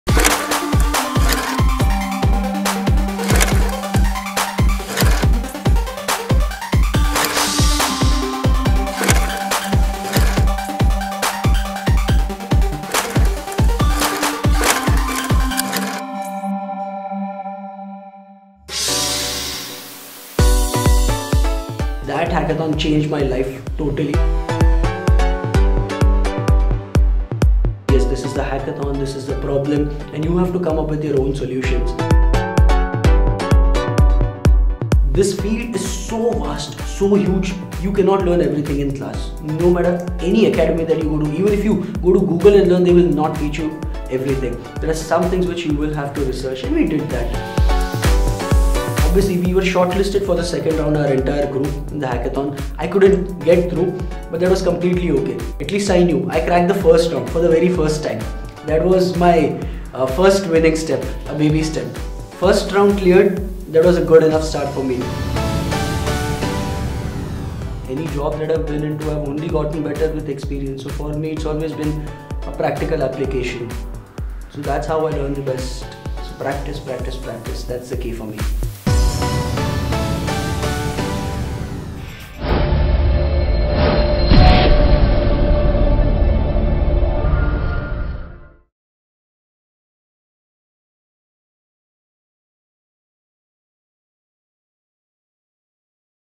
GreyAtom | My First Hackathon | Data Science | Sagar Dawda Story
Get access to FREE Data Science courses, projects, e-books, and more...

GreyAtom | My First Hackathon | Sagar Dawda Story

Hackathon is an integral part of the curriculum of GreyAtom school of data science. Hackathons are more than a regime, they play a crucial role in building a successful data scientist. For the learners, Hackathon can be a life-altering event which gives direction to their career.

This is the story of one such learner. GreyAtom's alumni Sagar Dawda shares the story of his first Hackathon and how the event opened new doors for him.